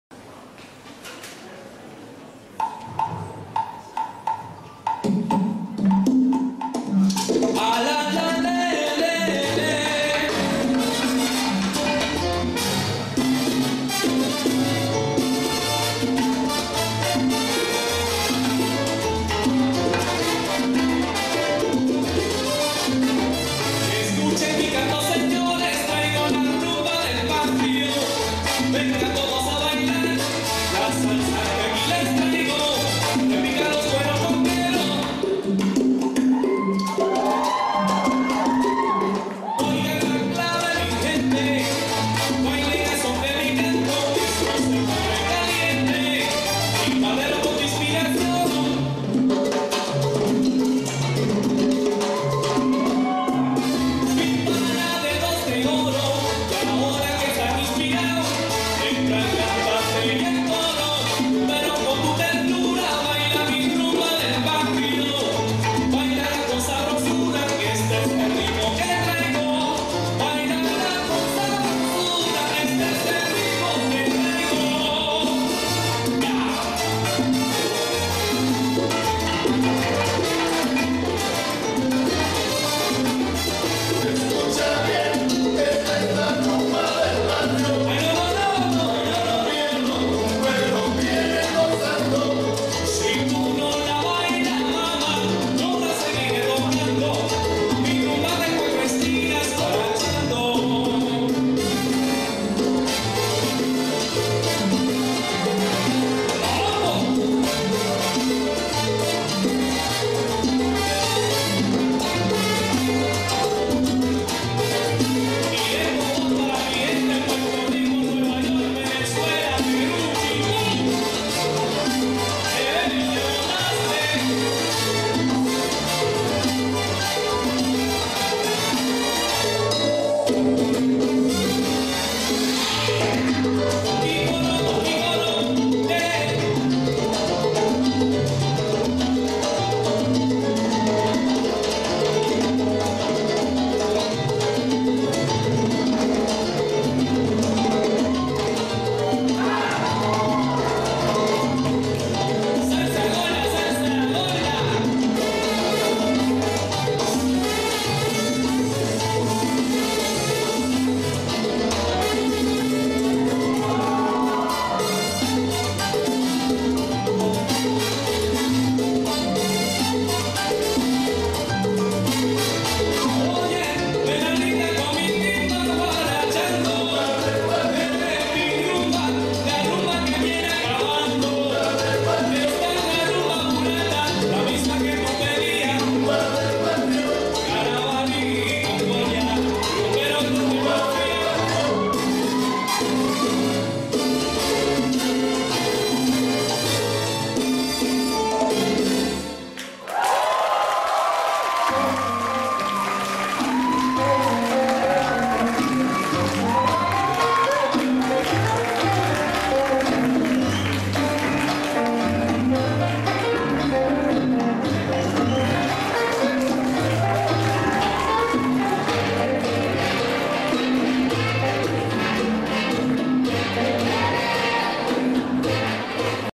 2nd China Salsa Festival, April 28 - Bailalo D.C. Performance
2nd China Salsa Festival, April 28 - Bailalo Dance Studio Performance